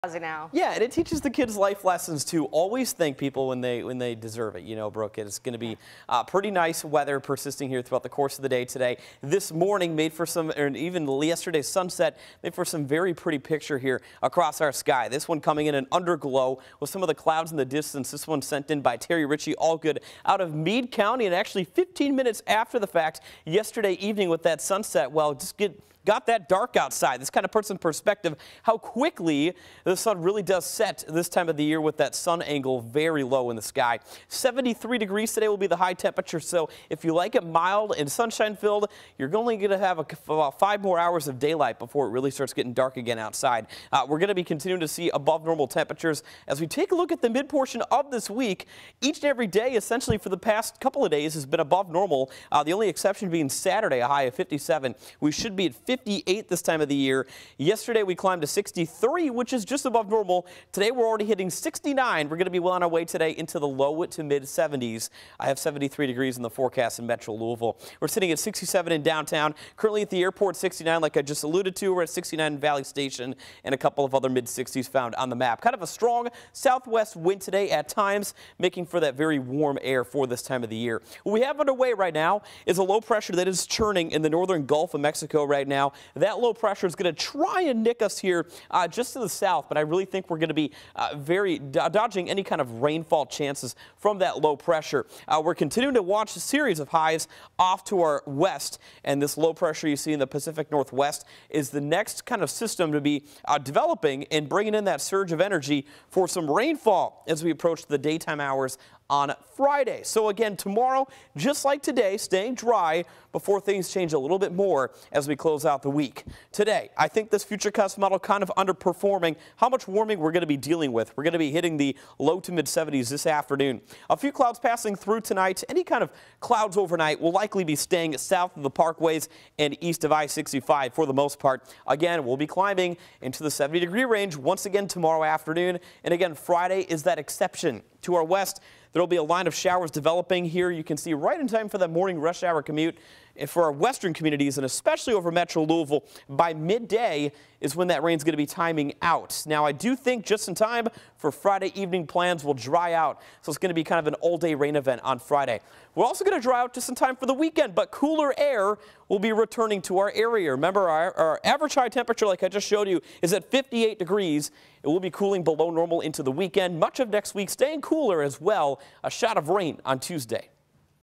Warm air sticks around Wednesday | Nov. 15, 2023 WHAS11 Noon Weather
We are picking up warmer than normal temperatures and sunshine both Wednesday and Thursday! Rain moves back into Kentuckiana on Friday.